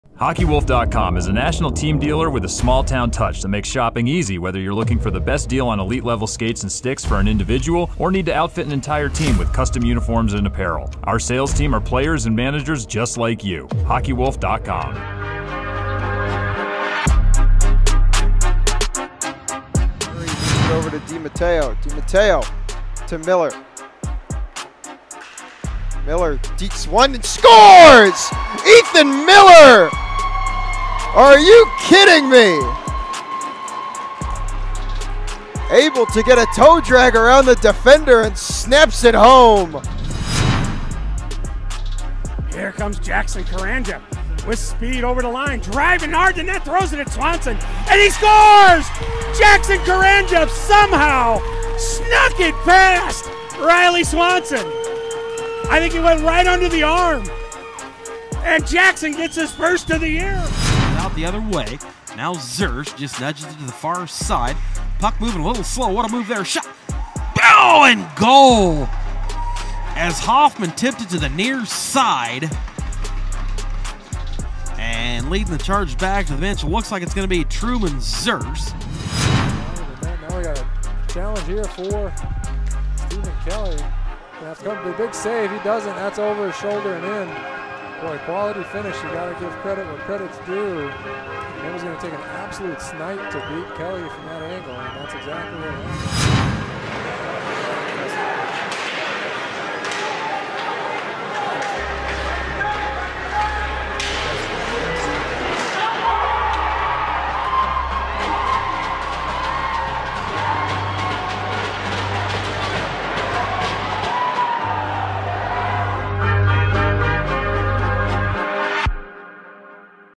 NA3HL Top Plays - September 30 - October 6, 2024
The top plays in the NA3HL September 30 - October 6, 2024, presented by Hockeywolf

5. Binghamton Buzz
4. St. Louis Jr. Blues
3. Wausau Cyclones
2. West Bend Power
1. Alexandria Blizzard